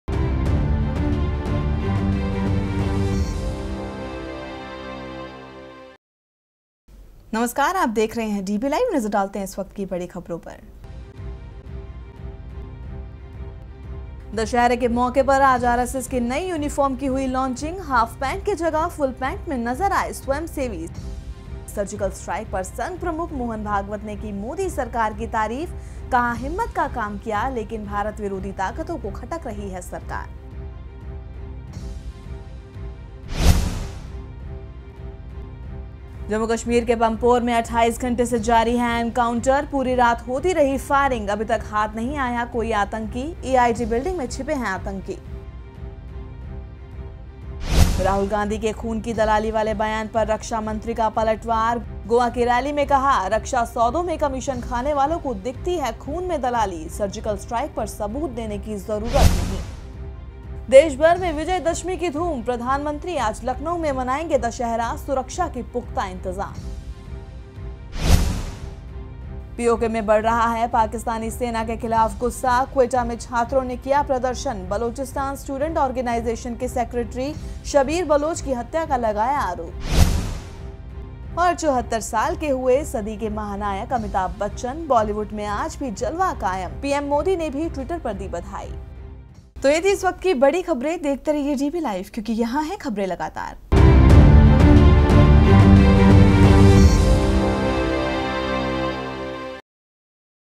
DBLIVE | 11 October 2016 | News Headlines | मुख्य सुर्खियां
..दशहरा के मौके पर आज आरएसएस की नई यूनीफॉर्म की हुई लॉन्चिंग...हाफ पेंट की जगह फूल पेंट में नज़र आए स्वंय सेवी...सर्जिकल स्ट्राइक पर संघ प्रमुख भागवत ने की मोदी सरकार की तारीफ... कहा, हिम्मत का काम किया, लेकिन भारत विरोधी ताकतों को खटक रही है सरकार।
मोदी सरकार को संघ की शाबाशी

2..जम्मू-कश्मीर के पंपोर में 28 घंटे से जारी है एनकाउंटर...पूरी रात होती रही फायरिंग...अभी तक हाथ नहीं आया कोई आतंकी...ईडीआई बिल्डिंग में छिपे हैं आतंकी।
28 घंटे से पंपोर में एनकाउंटर जारी
3..राहुल गांधी के खून की दलाली वाले बयान पर रक्षा मंत्री का पलटवार...गोवा की रैली में कहा, रक्षा सौदों में कमीशन खाने वालों को दिखती है खून में दलाली...सर्जिकल स्ट्राइक पर सबूत देने की ज़रूरत नहीं।
राहुल को पर्रिकर का जवाब
4..देशभर में विजयदशमी की धूम..प्रधानमंत्री आज लखनऊ में मनाएंगे दशहरा...सुरक्षा के पुख्ता इंतज़ाम
मोदी की ‘खास’ विजयदशमी
5..पीओके में बढ़ा रहा है पाकिस्तानी सेना के खिलाफ गुस्सा...क्वेटा में छात्रों ने किया प्रदर्शन...बलोचिस्तान स्टूटेंड ओर्गेनाइजेशन के सेक्रेटी शबीर बलोच की हत्या का लगाया आरोप।
पाक सेना के ख़िलाफ प्रदर्शन तेज़
6..74 साल के हुए सदी के महानायक अमिताभ बच्चन...बॉलीवुड में आज भी जलवा कायम..पीएम मोदी ने भी ट्वीटर पर बधाई दी।
74 के हुए शहंशाह